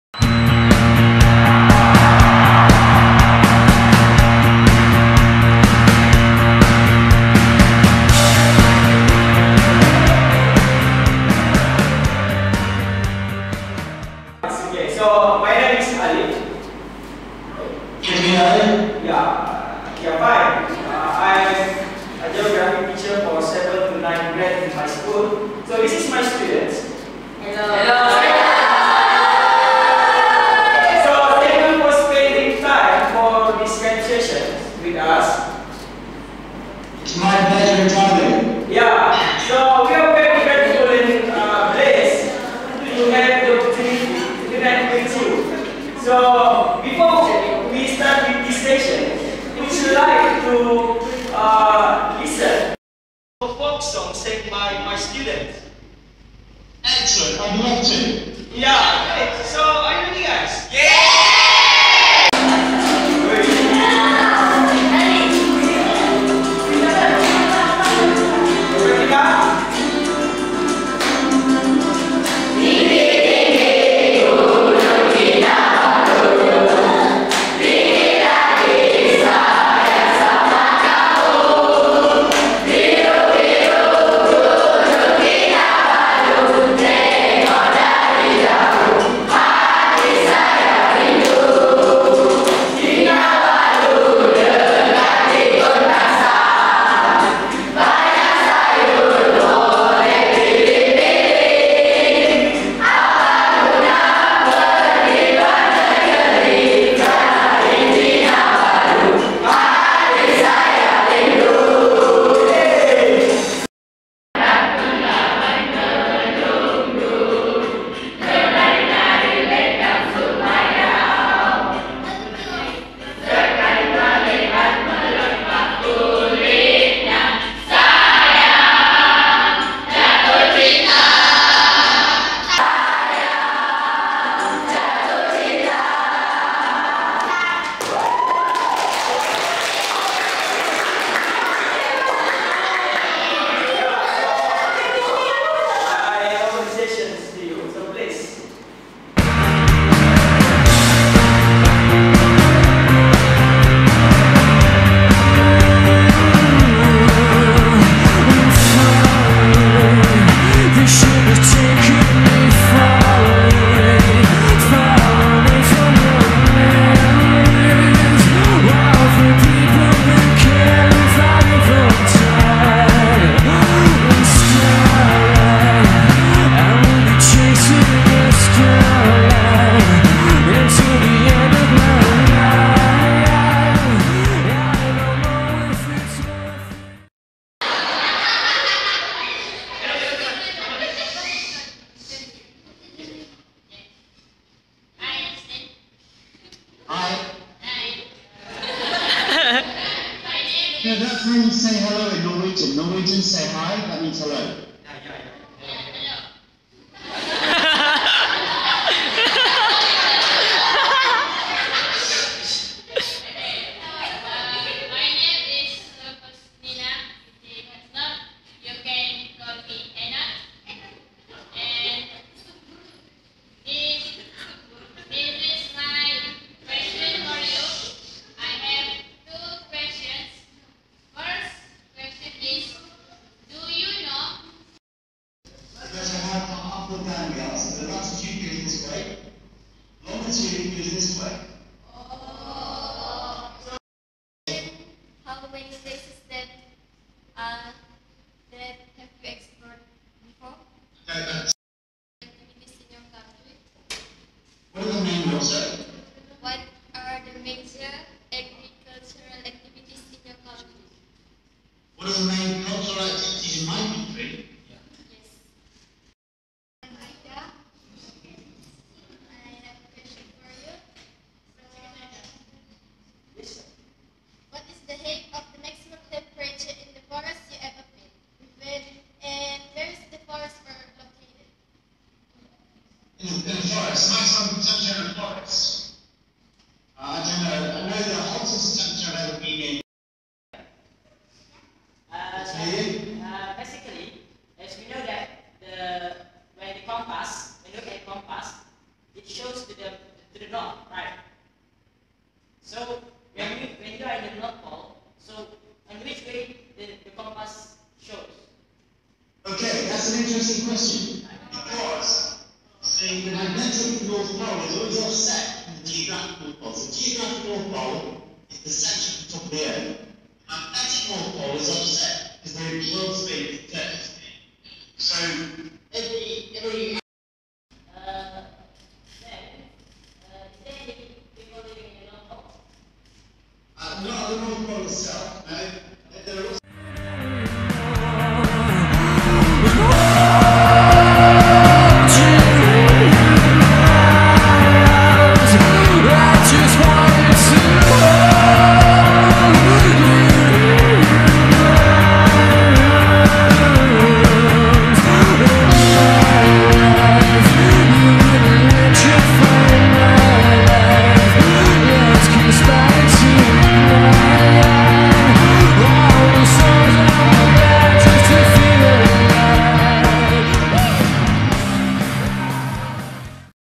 Mrsm Tun Mohamamd Fuad Stephens SkypeLesson with Justin Miles
MRSM Tun Mohammad Fuad Stephens Skype session with Justin Miles. Title : Exploring with an explorer.